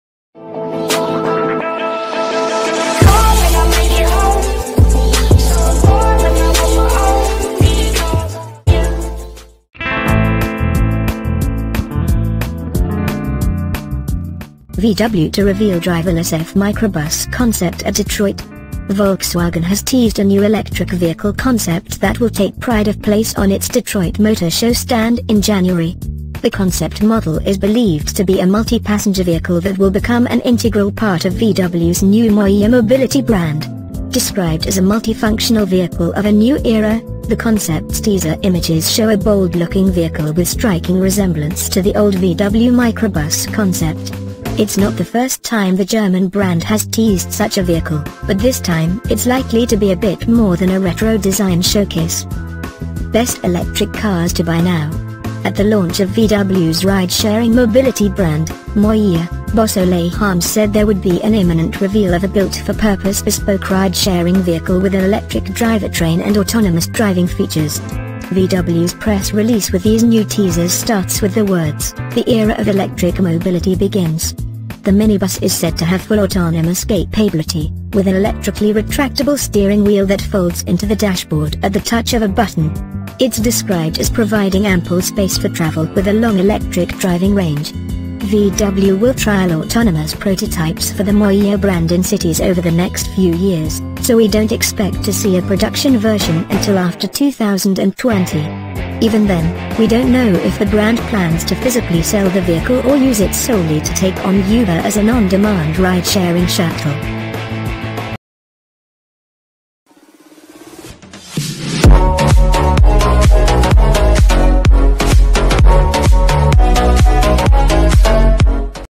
[NEWS] VW To Reveal Driverless EV Microbus Concept
VW to reveal driverless EV 'microbus' concept at Detroit. Volkswagen has teased a new electric vehicle concept that will take pride of place on its Detroit Motor Show stand in January. The concept model is believed to be a multi-passenger vehicle that will become an integral part of VW's new MOIA mobility brand.

Described as a "multi-functional vehicle of a new era", the concept's teaser images show a bold-looking vehicle with striking resemblance to the old VW Microbus concept. It's not the first time the German brand has teased such a vehicle, but this time it's likely to be a bit more than a retro design showcase.

At the launch of VW's ride-sharing mobility brand, MOIA, boss Ole Harms said there would be an "imminent reveal" of a "built for purpose" bespoke ride-sharing vehicle with an electric drivetrain and autonomous driving features. VW's press release with these new teasers starts with the words, "the era of electric mobility begins".

The minibus is said to have full autonomous capablity, with an electrically-retractable steering wheel that folds into the dashboard at the touch of a button. It's described as providing "ample space for travel with a long electric driving range".

VW will trial autonomous prototypes for the MOIA brand in cities over the next few years, so we don't expect to see a production version until after 2020. Even then, we don't know if the brand plans to physically sell the vehicle or use it solely to take on Uber as an on-demand ride-sharing shuttle.